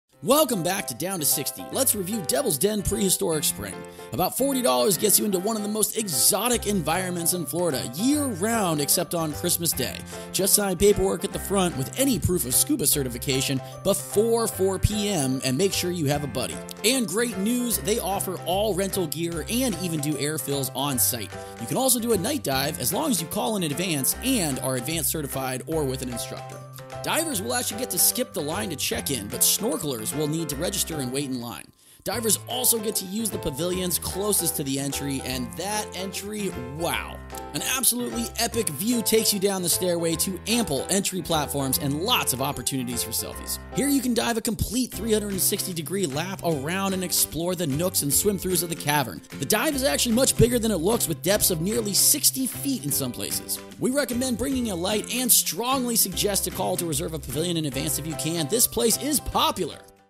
Devils Den Scuba Diving Quick Facts - Down to 60
Devil's Den Prehistoric Spring is built around the scuba diver, but it's also great for snorkelers.  Diver's have a lot of equipment to carry, so for safety they are given priority on the walkway and entry areas.
Divers get a Fastpass to go around any lines straight to the check-in counter, snorkelers will need to register and wait in line.
Divers even get to use the pavilions closest to the entry.
An EPIC VIEW takes you down the stairway to ample entry platforms for pre dive setup and lots of opportunities of selfies.
The middle is like a hill, where you can dive a complete 360 degree lap around and explore the nooks and swimthroughs of the cavern. The dive is much bigger than it looks, with depths of nearly 60 feet in some places. It extends well beyond the waterline under out rock ledge.
72 degrees F year-round, and protection from the wind make this place a perfect winter time diver refuge.

Although no cave diving is allowed, we do recommend a light.  You only need to be Open Water Certified to do this dive, and this place is super popular for dive classes as well.  And great news, they do tank fills, gear rentals, and have a store with all those last-minutes necessities, so that little thing you forgot won't ruin your trip or class.

About $40 per diver gets you in, but make sure you sign in with a dive buddy that you actually came with (can't find a random stranger and ask them to join, we've seen that NOT work to a poor Russian tourist who didn't have a  Down to 60 to watch first.  You'll need some form of scuba certification (app is accepted) unless signing in with an instructor as part of a scuba course.  The last water entry is 1hr before closing (5pm).

We strongly suggest calling to reserve a pavilion in advance if you can, as well as the pool or cabins for overnight stays.  Instructors can rent the pool for teaching scuba lessons at an hourly rate, but it's reservations only so be sure to call ahead.
They're open every day of the year except Christmas.  Santa Claus and the Devil don't like each other, but rumor has it they're dive buddies (which seems odd, even to us).

PS:  Devil's Den is only 5 minutes away from Blue Grotto, in the middle of Florida - so they pair nicely for a day trip of diving or a scuba course.

Voice by Jonny Green
Towel Guy:  Jonny Green
Written and Edited by Kenneth Dyal